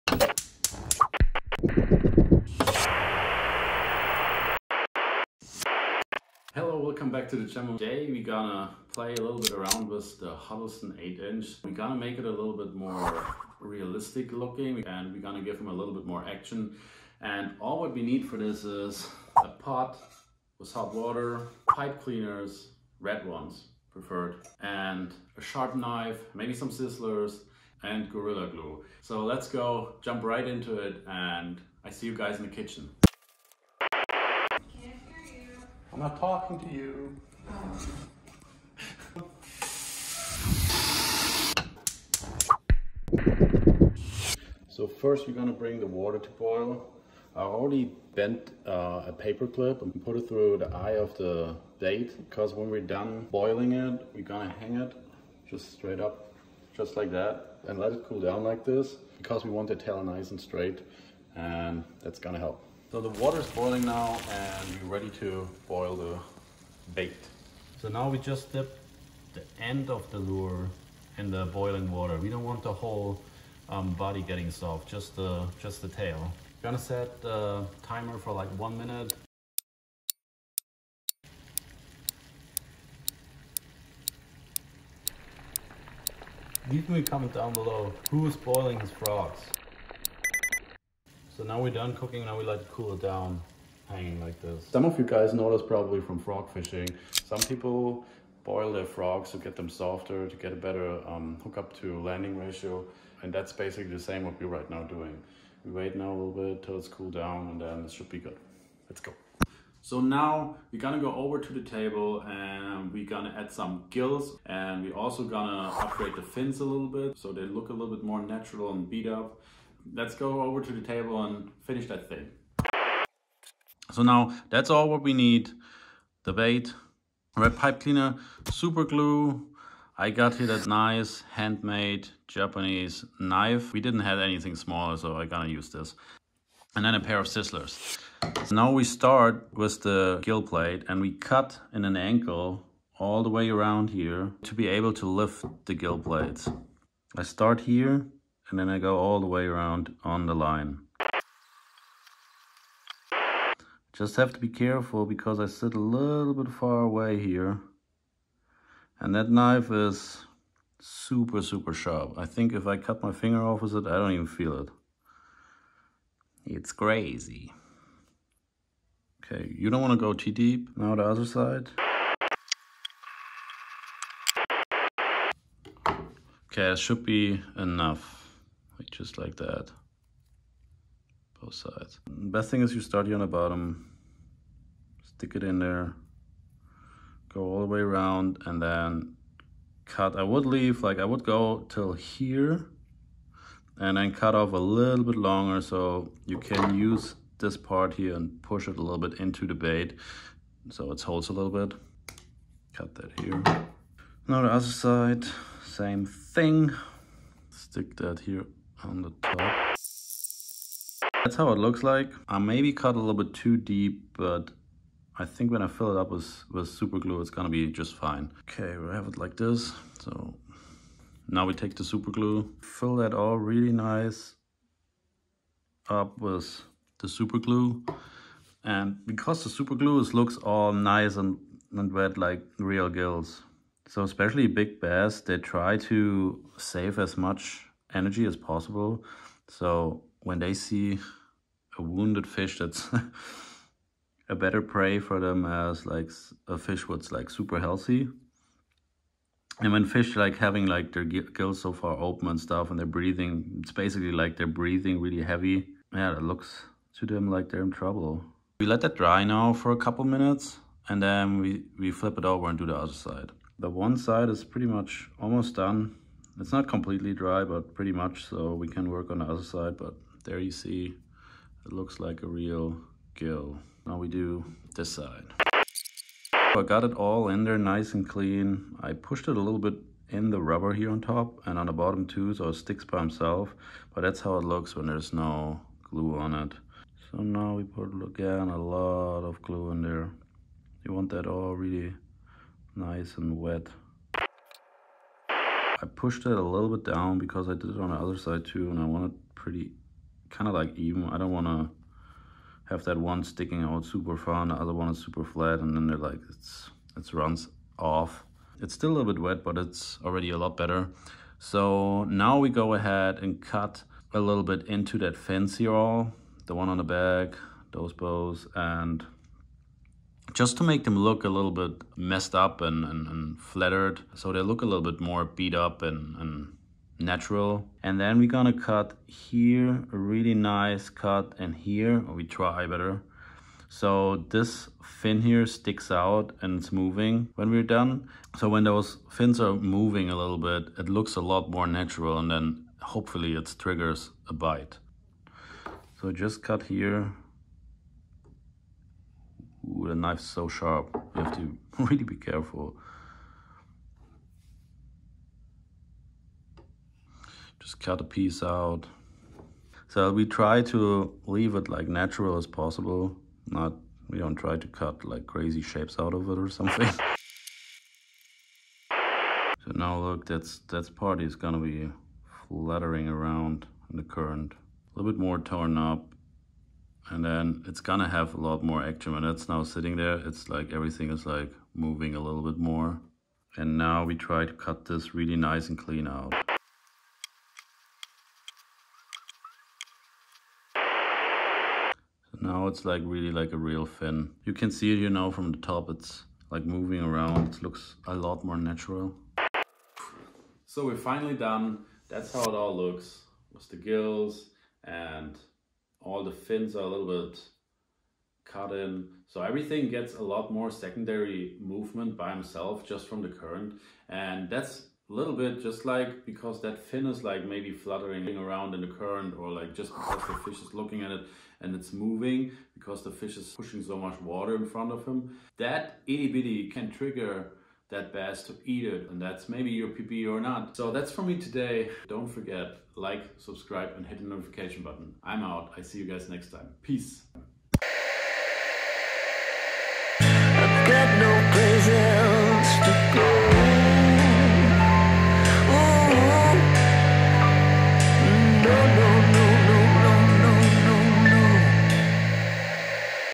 TOP 3 Huddleston Swimbait Modifications You Need To Know!
My top 3 modifications for a Huddleston swimbait. Add gills, modify the fins, and a softer tail for a more life-like look and more action even in the coldest water.

Go Follow!

Bait used: Huddleston Deluxe 8" color: Hitch

8901 Tehama Ridge Pkwy, Ste 127, PMB 248.